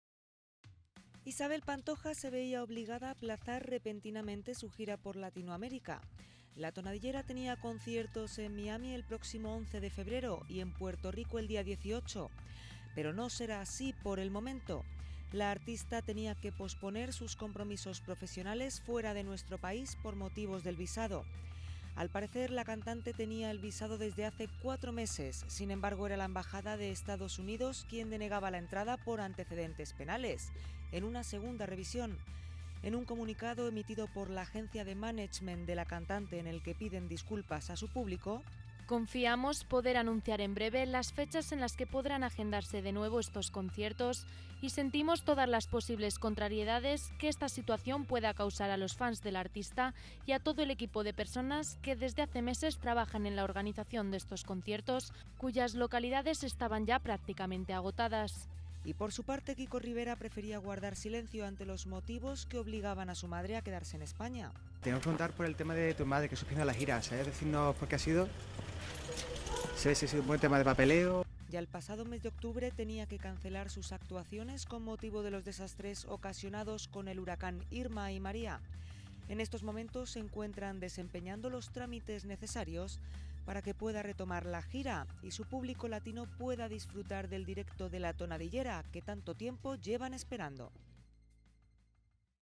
Isabel Pantoja no puede viajar por problemas con el visado
Isabel Pantoja se ha visto obligada a aplazar repentinamente su gira por Latinoamérica. La tonadillera tenía conciertos en Miami el próximo 11 de febrero y en Puerto Rico el día 18, sin embargo no será así, por el momento. La artista se ha visto obligada a posponer sus compromisos profesionales fuera de nuestro país por motivos del visado. Al parecer la artista tenía el visado desde hace cuatro meses sin embargo la era la embajada de Estados Unidos quien denegaba la entrada por antecedentes penales, en una segunda revisión.